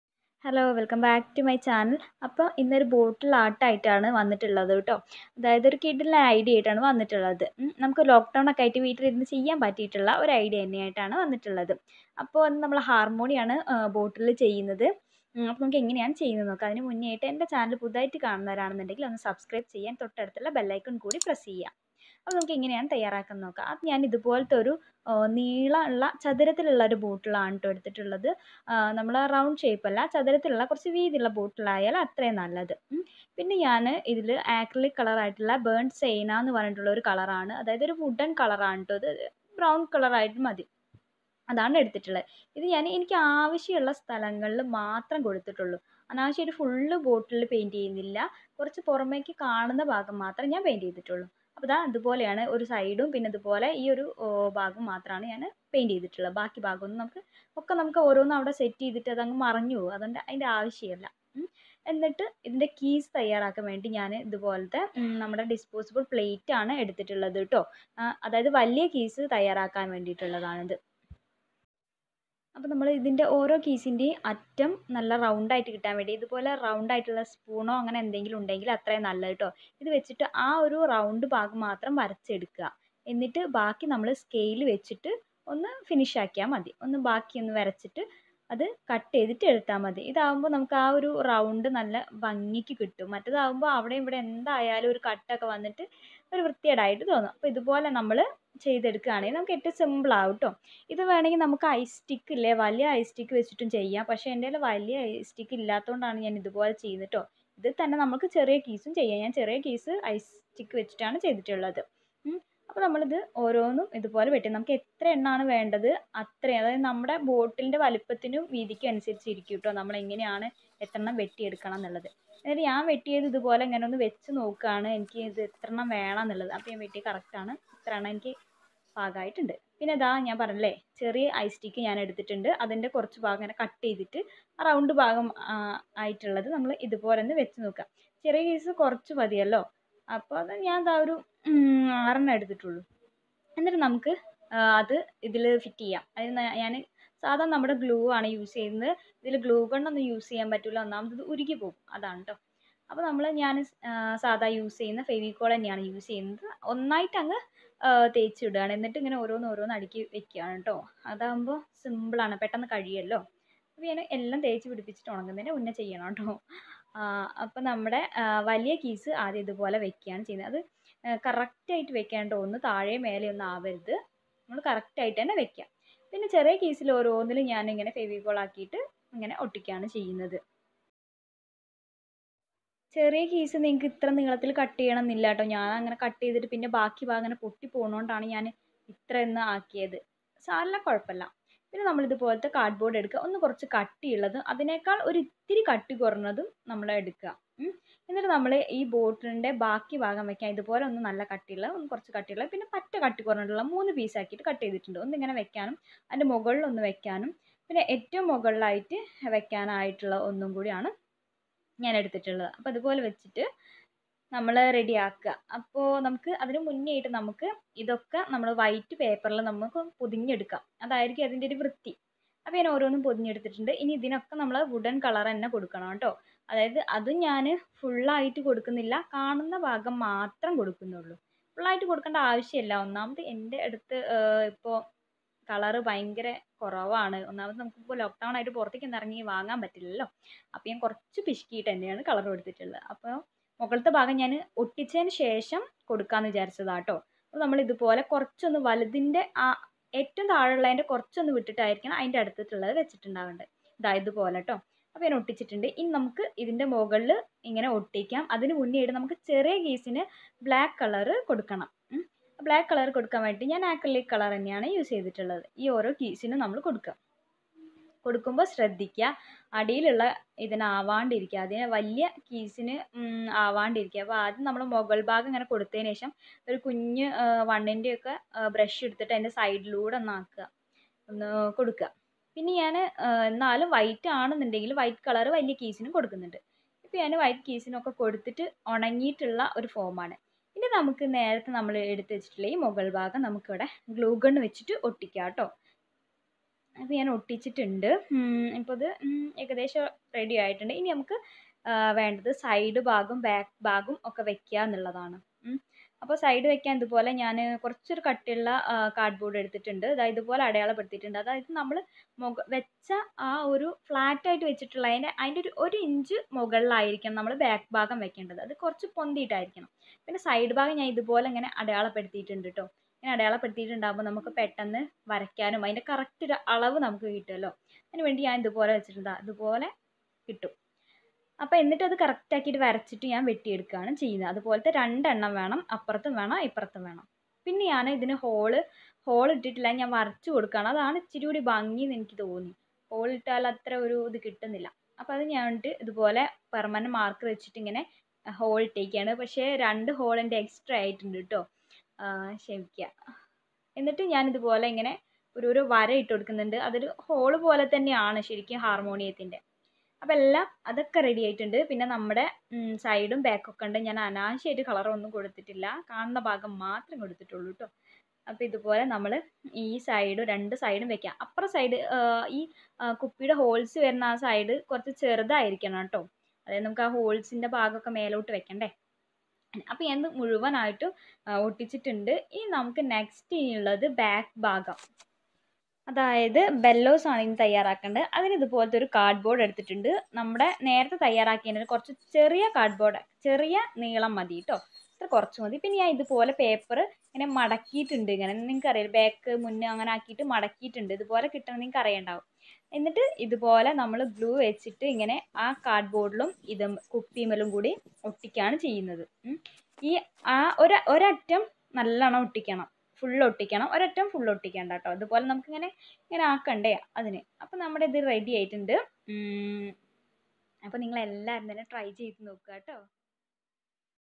Harmonium bottle art | DIY bottle art malayalam
Hi frndzzz...😍😍
New one is redy..  harmonium on bottle.. must watch and try this...